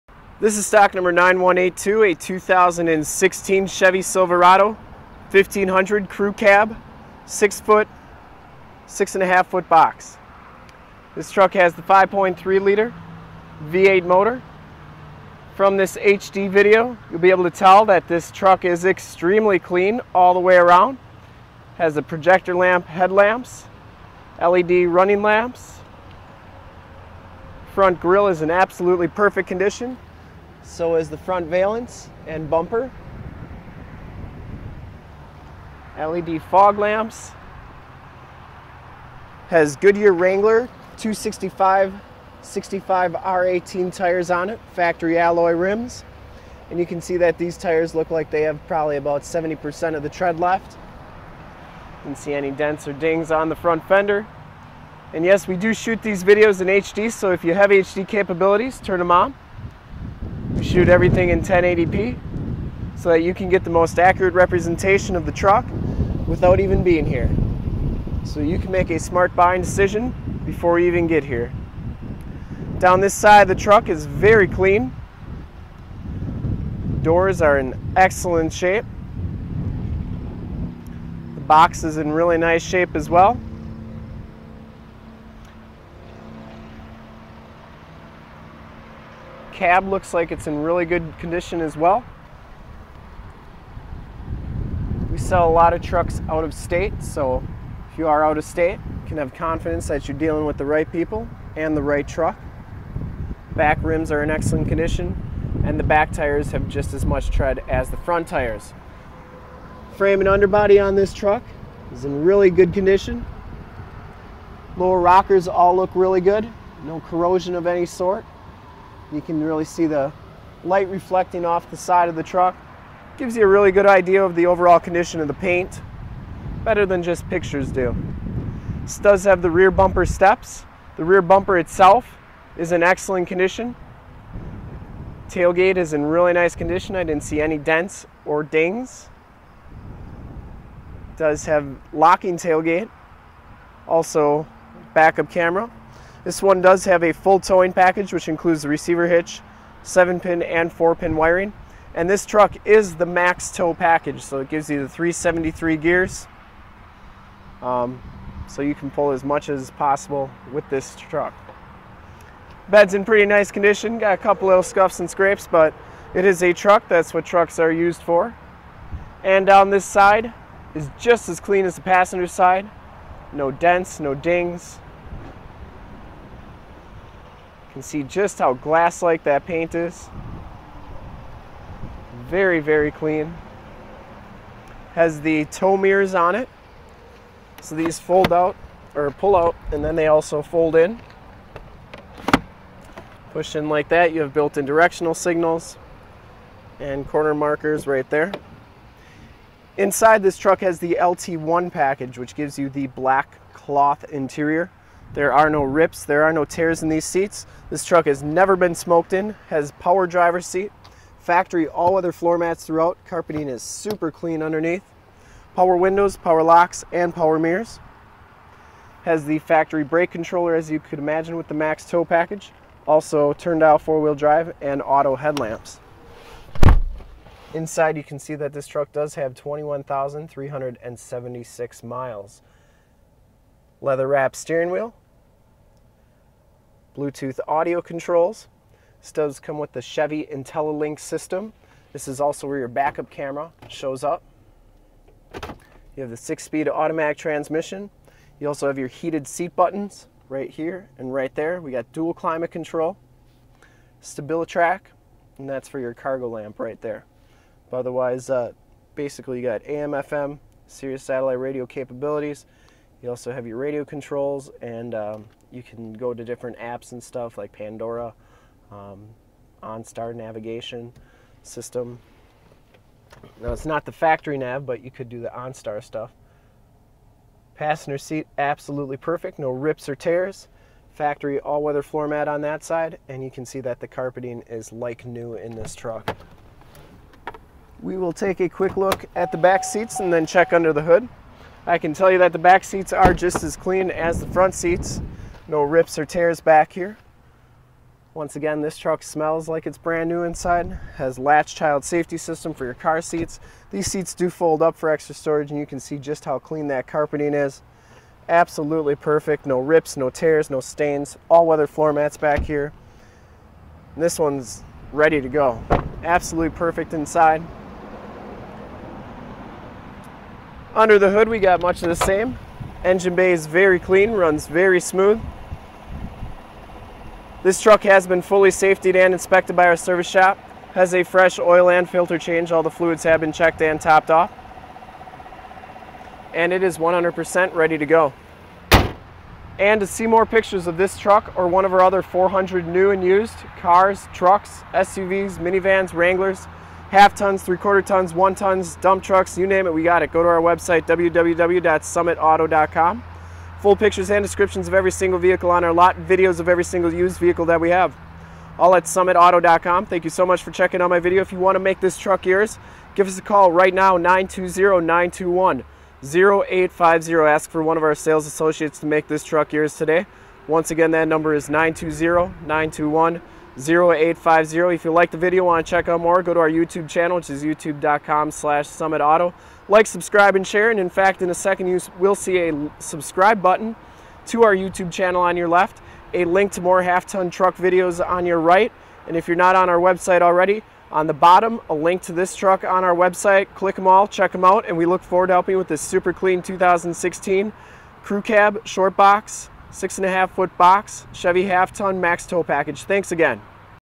2016 CHEVROLET SILVERADO 1500 MAX TOW PACKAGE WISCONSIN WALK AROUND REVIEW SOLD! 9182 SUMMITAUTO
This 2016 CHEVROLET SILVERADO 1500 MAX TOW PACKAGE LT1 IN BLACK CLEARCOAT is the vehicle we did walk around review of today.

2016 CHEVROLET SILVERADO 1500 MAX TOW PACKAGE LT1 IN BLACK CLEARCOAT

STOCK: 9182
PRICE: $35,999
MILES: 21,366
MAKE: CHEVROLET
MODEL: SILVERADO 1500
VIN: 3GCUKREC0GG328186

1 Owner! Clean Title History! 5.3 Liter V8 Engine, CREW, SHORT 6 1/2 FT BOX, LT1, MAX TRAILER TOW PKG, TOW MIRRORS, BACKUP CAM, BLACK CLOTH SEATS, POWER DRIVERS SEAT, 40/20/40 SPLIT BENCH SEATING, with E85 Capabilities FFV Flex Fuel Vehicle, Active Fuel Management, 355 Horsepower, Full Four Door Crew Cab, Short Box 6 1/2 Foot Shortbox, LT1 Package 1LT LT, 6 Speed Automatic Transmission with Optional Manual Tap Shift, Turn Dial 4x4 Four Wheel Drive 4WD, Reverse Backup Camera Rearview Camera, Onstar System, Power Driver's Seat, Dual Heated Seats, Non Smoker, Black Ebony Cloth Seats, 40/20/40 Split Front Bench Seating with Center Seat Hidden Storage Compartment, Full Towing Package with Receiver Trailer Hitch, Wiring and Transmission Cooler Tow Package, Factory Brake Controller, Telescopic Tow Power Mirrors with Built-in Directional Signals, Stabilitrak Traction Control, 3.73 Gears with Automatic Locking Differential Limited Slip Differential, Goodyear Wrangler SR-A P265/65 R18 Tires, Factory Alloy Rims Premium Wheels, Four Wheel Disc Brakes, Bedrail Covers, Bug Shield, Fog Lights, EZ Raise Assist Tailgate, Locking Tailgate, Rear Bumper Steps, Chevy MyLink Touchscreen Radio, AM / FM Radio Tuner, Sirius/XM Satellite Radio Capabilities Sirius / XM, Keyless Entry with Factory Remote Start, Rear Window Defroster, Adjustable Height Seatbelts, Driver and Passenger Front Air Bags, L.A.T.C.H. Child Safety System, Side Curtain Air Bags SRS Safety Restraint System, Multi-Function Steering Wheel Controls, Compass, Outside Temperature Display and Mileage Display, Dual Multi-Zone Climate Control , Factory All Weather Floormats, Air Conditioning AC, Cruise Control, Power Locks, Power Windows, Automatic Headlights Autolamp, Tilt/Telescope Steering Wheel, 3 Year / 36,000 Mile Remaining Factory Bumper to Bumper Warranty, Whichever comes first, Black, One Owner! Clean Autocheck! Very very clean inside and out! This is one of the sharpest 2016 Chevrolet Silverado 1500 crewcab shortbox 1/2 ton trucks on our lot! Make your move before this super rare ride is gone!

STOCK: 9182
PRICE: $35,999
MILES: 21,366
MAKE: CHEVROLET
MODEL: SILVERADO 1500
VIN: 3GCUKREC0GG328186